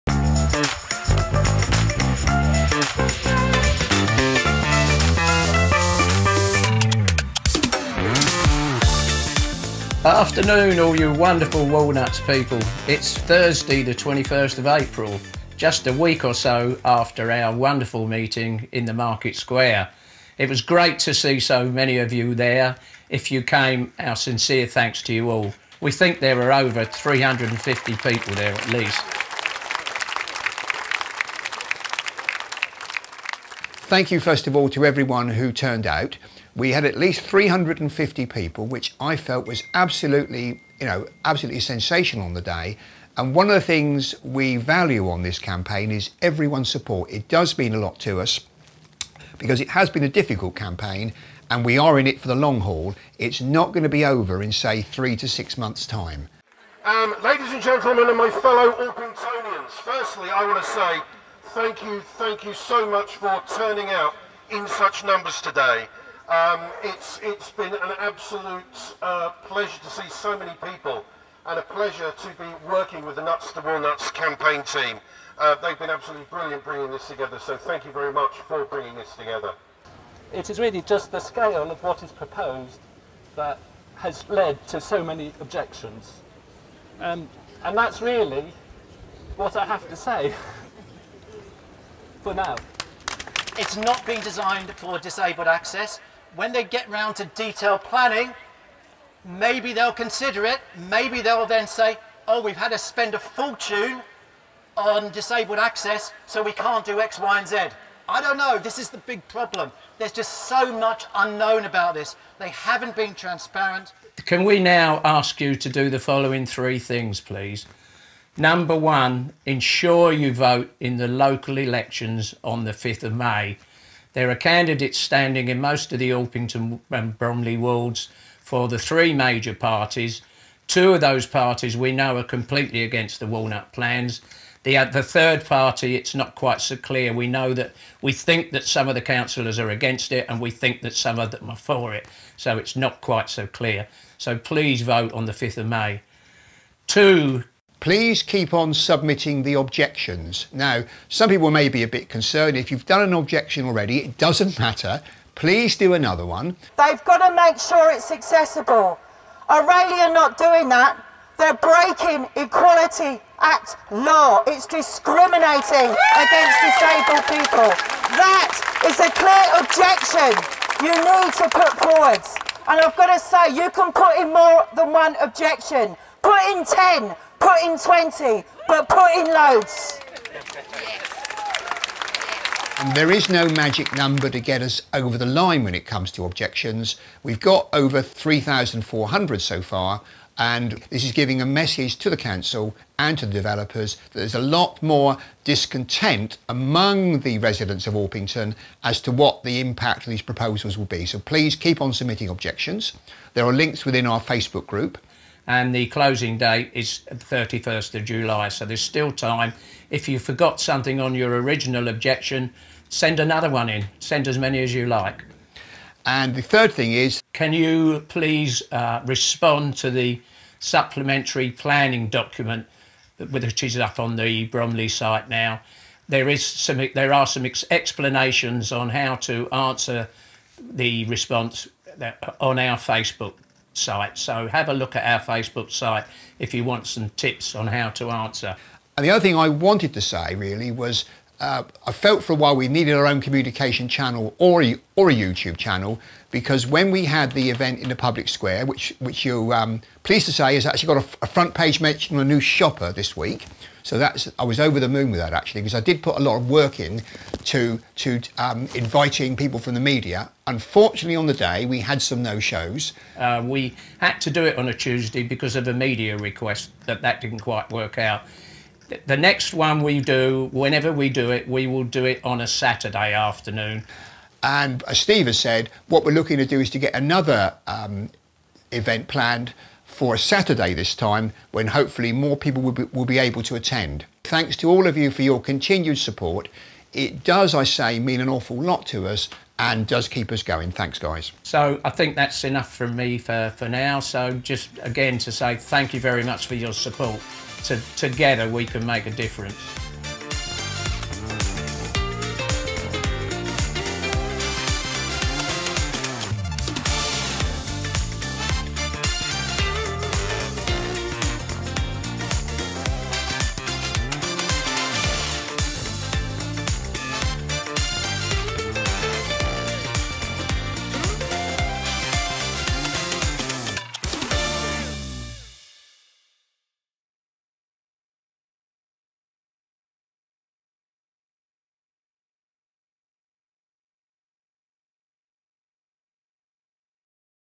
Nuts to the Walnuts - highlights of Public Meeting plus interviews with Steve & Tim.
Tim and Steve are part of the Nuts To The Walnuts group. They are opposed to the proposed building of 15 high-rise residential/retail block in the centre of Orpington. This is a short edit with highlights of the public meeting and follow up talking heads from Tim and Steve.

Filmed and edited by James Rose Photography and Video